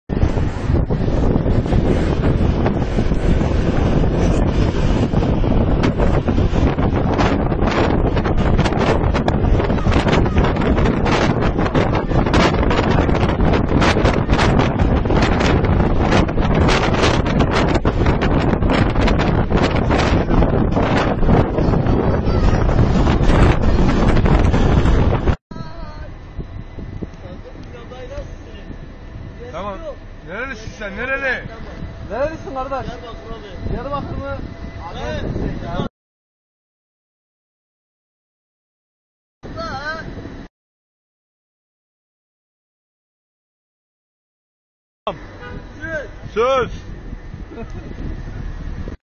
Tramvayda tehlikeli yolculuk kamerada
Beyoğlu ve Fatih'te tramvaya tutunup yolculuk yaparak hayatlarını tehlikeye atan çocuklar, çevredekiler tarafından cep telefonu kamerasıyla görüntülendi.

Beyoğlu Kabataş'ta iki çocuk hızla giden tramvayın arkasına tutunarak yolculuk yaptı. Çocuklardan birinin tramvay kapısına, diğerinin de arka cama tutunduğu dikkat çekti. Fatih'te ise 15 yaşlarında bir çocuk, tramvayın dikiz aynasına oturarak yolculuk yaptı. Cep telefonu ile kendisini görüntüleyen sürücüyle de konuşan çocuğun, rahat tavırları dikkat çekti.